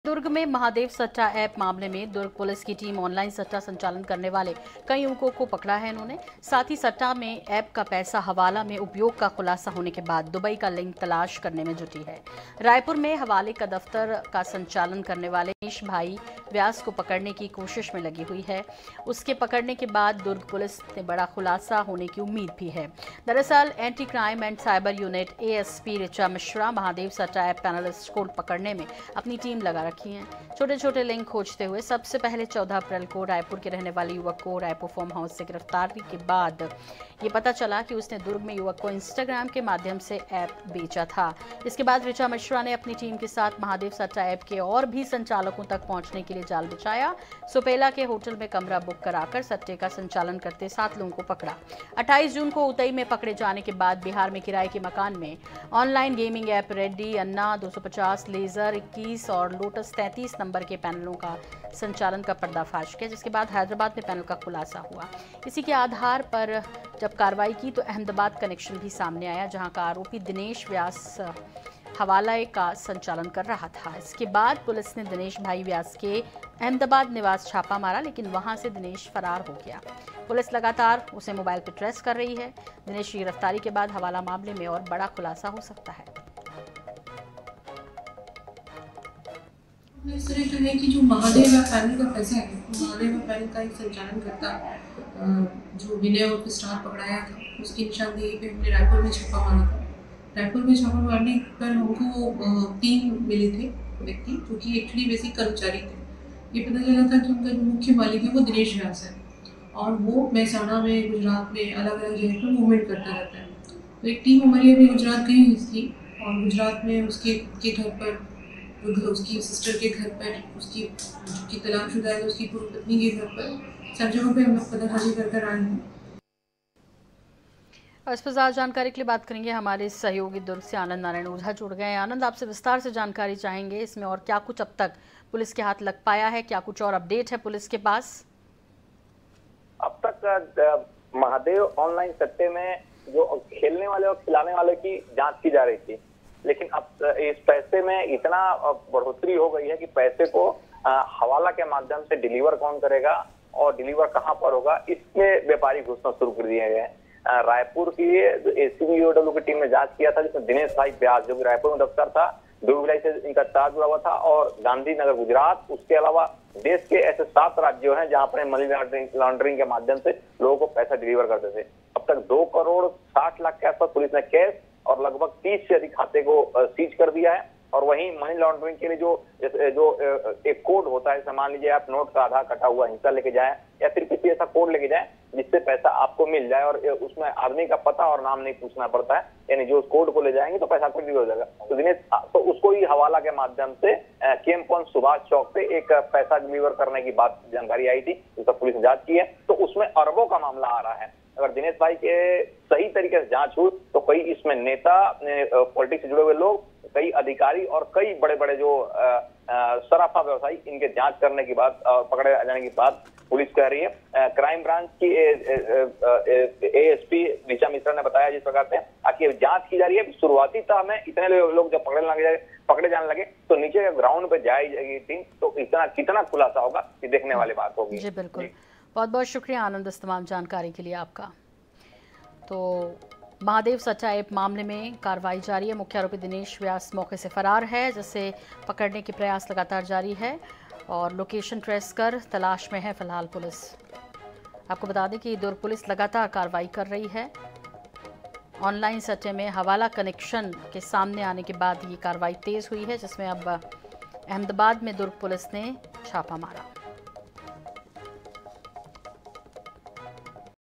Mahadev Satta Case: ऑनलाइन सट्टा में हवाला कनेक्शन भी आया सामने, Ahmedabad में Durg Police का छापा
INH, Mahadev Satta App Case: ऑनलाइन सट्टा में हवाला कनेक्शन भी आया सामने, अहमदाबाद में Durg Police का छापा

Incoming Search Terms: